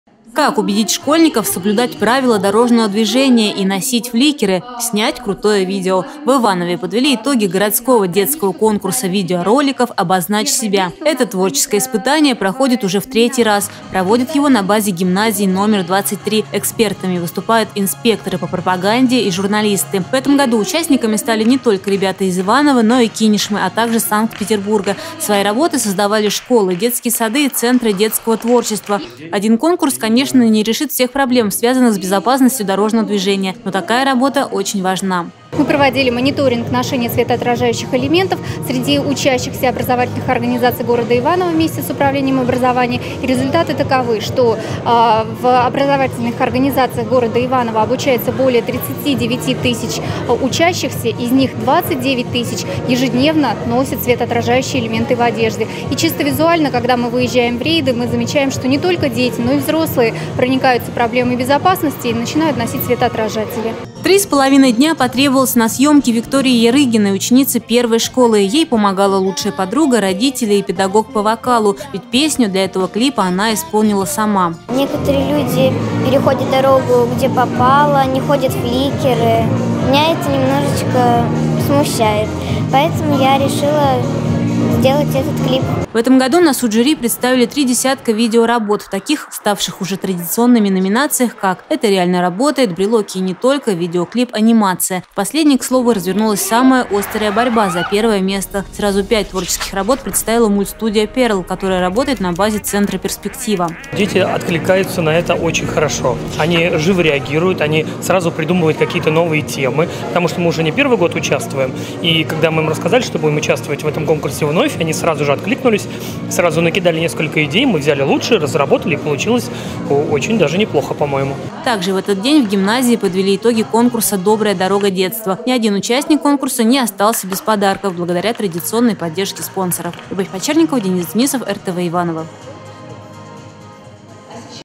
Обозначить себя – легко!
Как убедить школьников соблюдать правила дорожного движения и носить фликеры? Снять крутое видео. В Иванове подвели итоги городского детского творческого конкурса видеороликов «Обозначь себя».